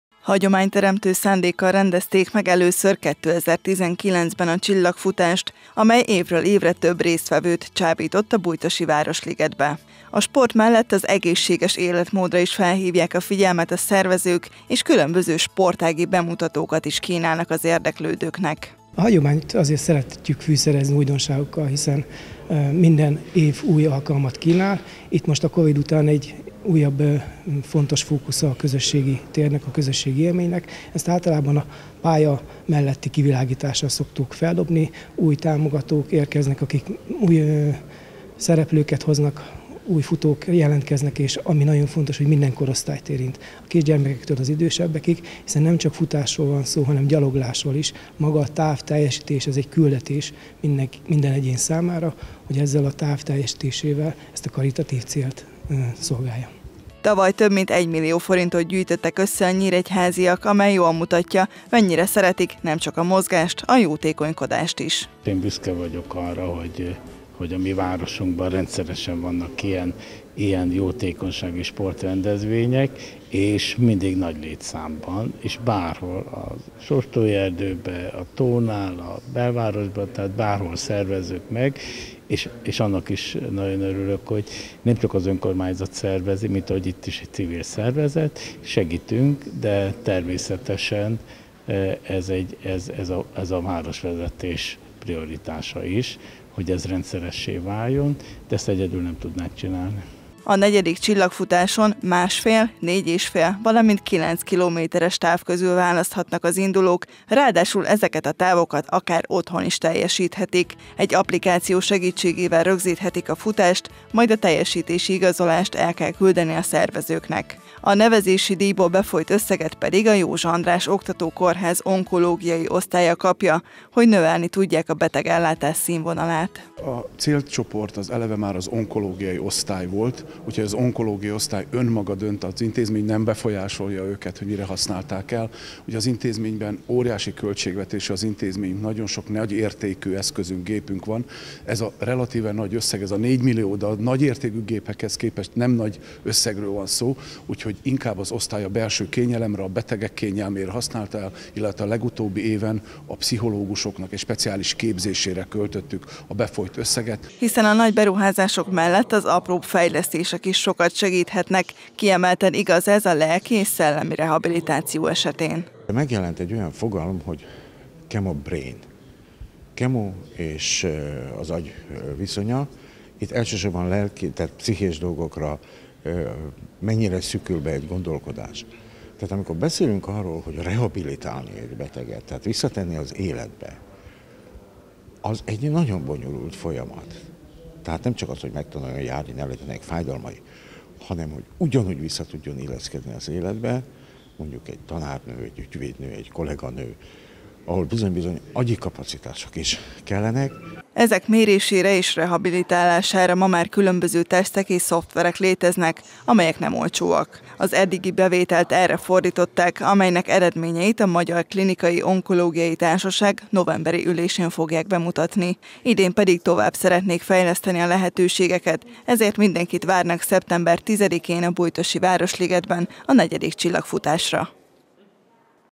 Jótékonykodás sporttal
Közel ezer résztvevője volt tavaly a Csillagfutásnak Nyíregyházán. A jótékonysági sporteseményt idén is megrendezik a Bujtosi Városligetben, a bevételt pedig a nyíregyházi Jósa András Oktatókórház Onkológiai osztályának ajánlják fel. A rendezvényen egyéb sportági bemutatókat is láthatnak az érdeklődők.